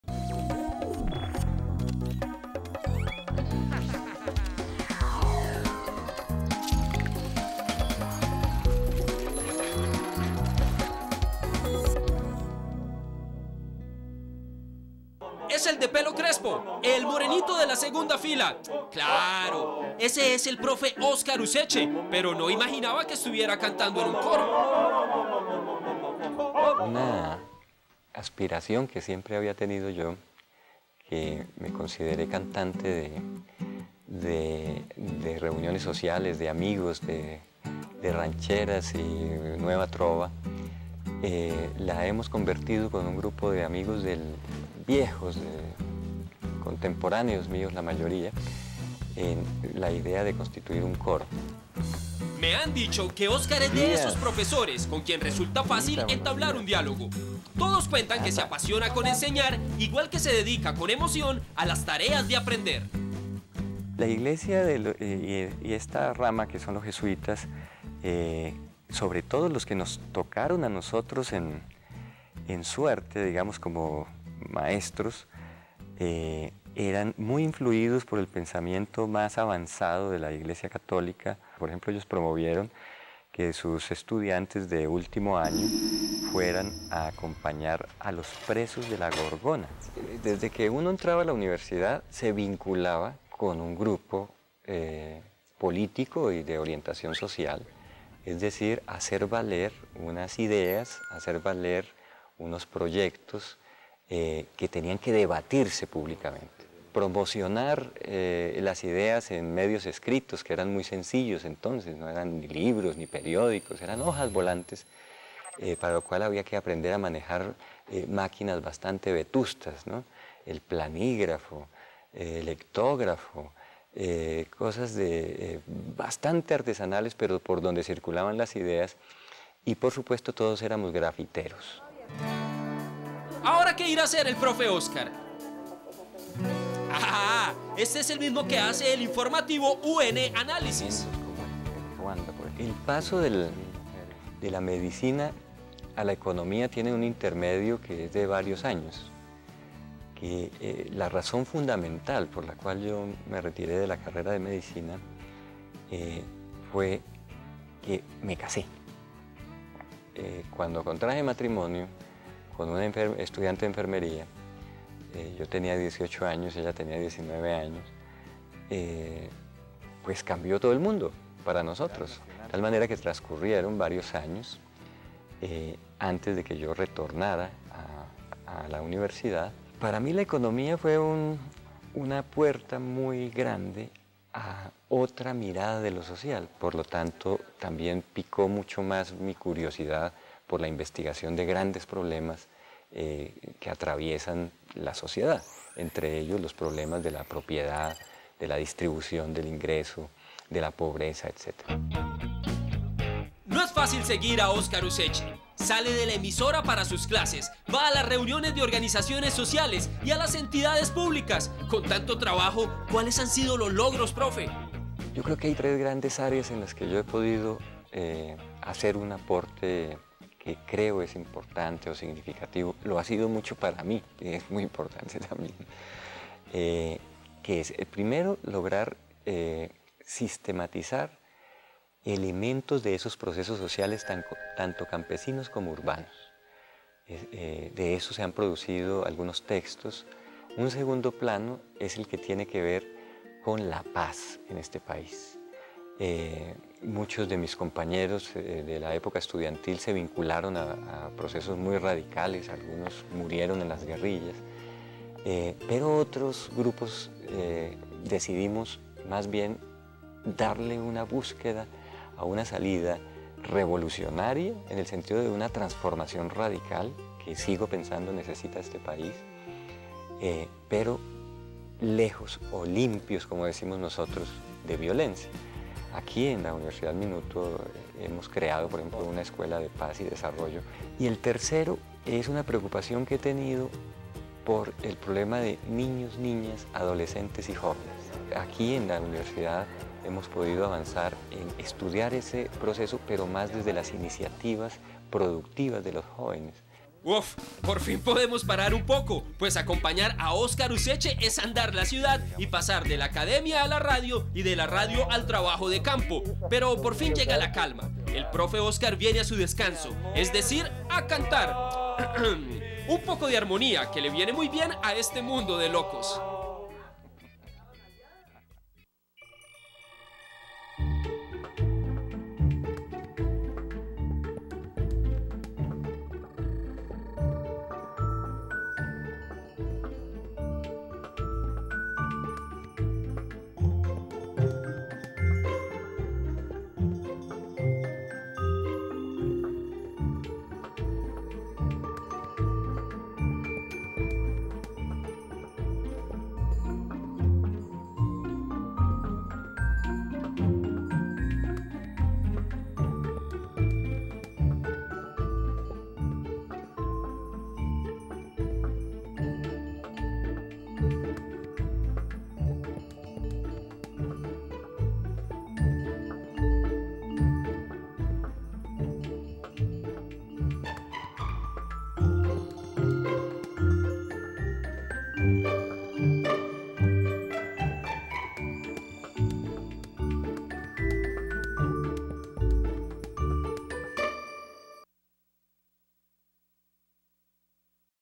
DE MENTES - Oscar Useche
Oscar Useche niño de intereses básicos pero no pasionales, se preguntaba como un avión al ser de gran tamaño y peso lograba volar, sin embargo esta clase de dudas no le significaron como a otros quizá, el deseo de algún día ser piloto o ingeniero; sus deseos de cambio y búsqueda de trasformación social a temprana edad fueron alimentados y avivados por los  movimientos  de cambio y lucha que a su época correspondieron.